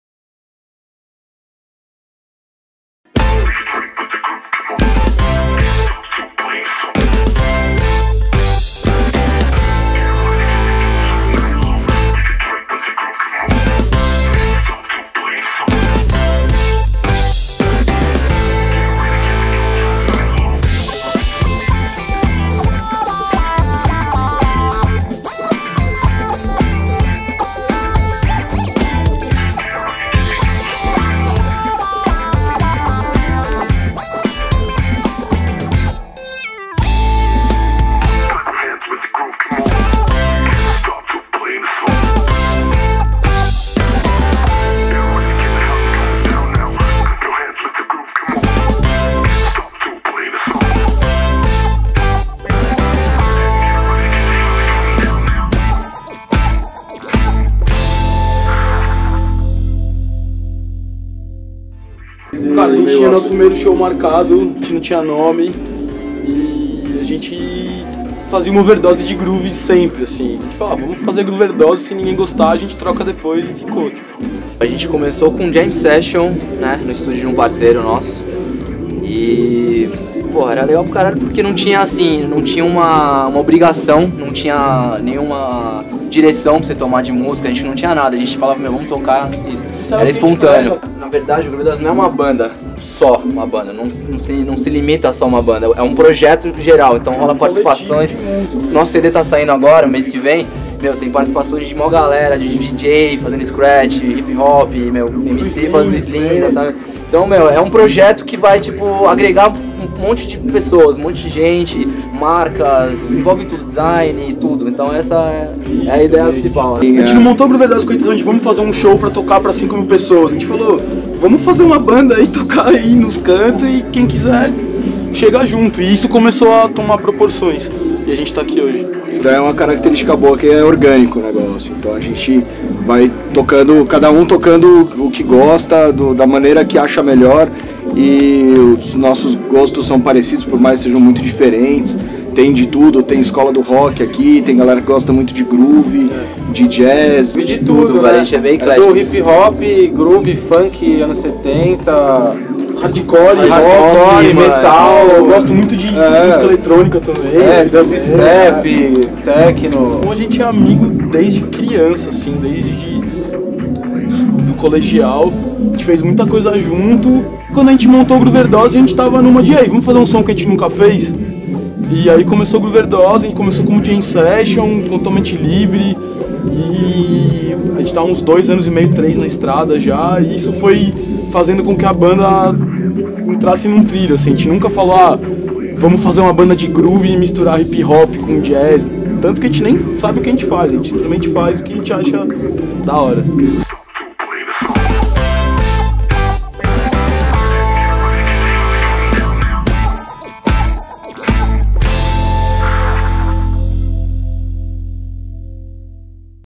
GROOVERDOSE - TEASER GOODFELLAS - HD
Teaser Clip e entrevista com a banda Grooverdose durante a GoodFellas Festa em 10/02/2012

FICHA TÉCNICA:
direção: Daniel Tupinambá
/cinegrafistas: Daniel Tupinambá, Marcel Campos, Juliana Lemes, Vinicius Camara, Pedro Gueller
/produção: Juliana Lemes
/operador de áudio: Gus Pereira
/edição e pós-produção: Augusto Moya
/agradecimentos: João Prado, Camila Tomo
/vinhetas: Maurício Kenzo